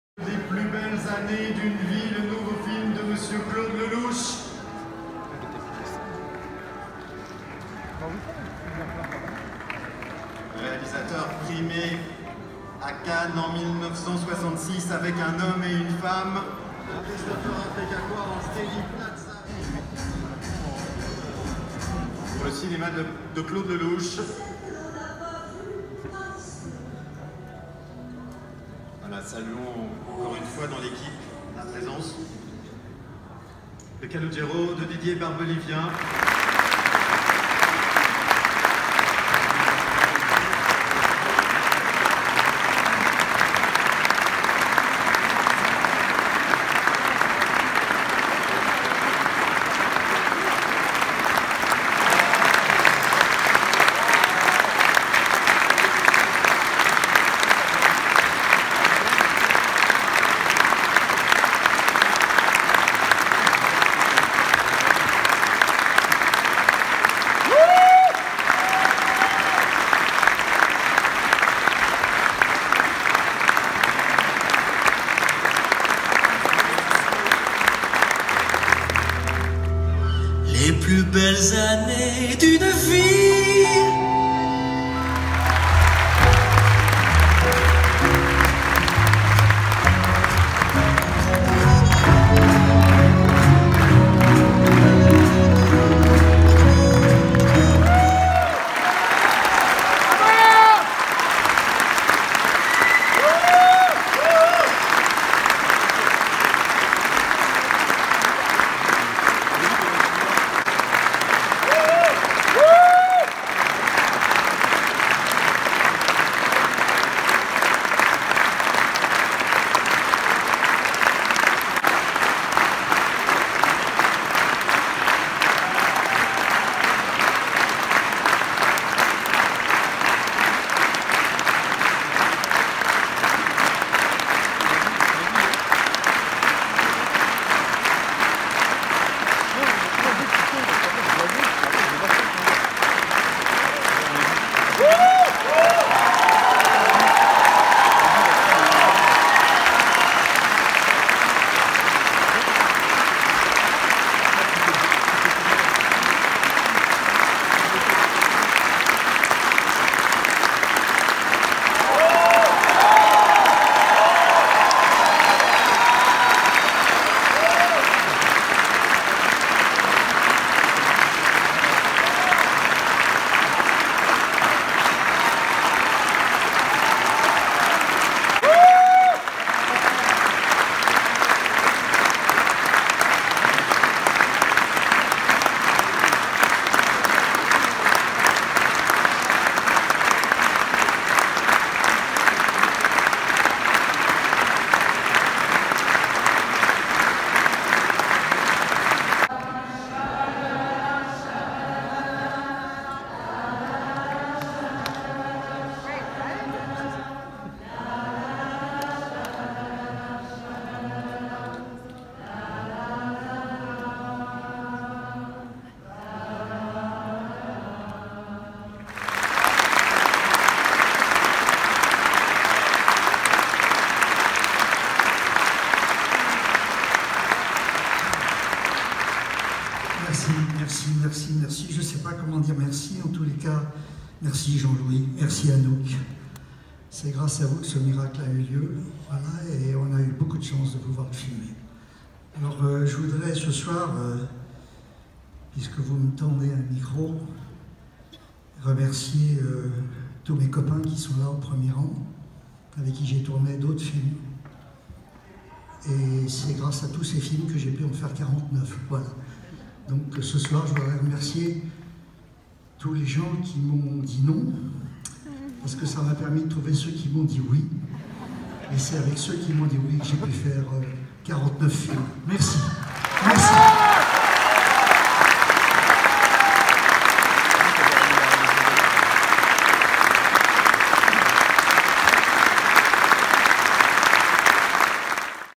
18/05/2019 Cannes : le triomphe de Lelouch
Petit bloc-notes vidéo cannois. Projection des plus belles années d'une vie dans le grand théâtre Lumière. Accueil triomphal du public !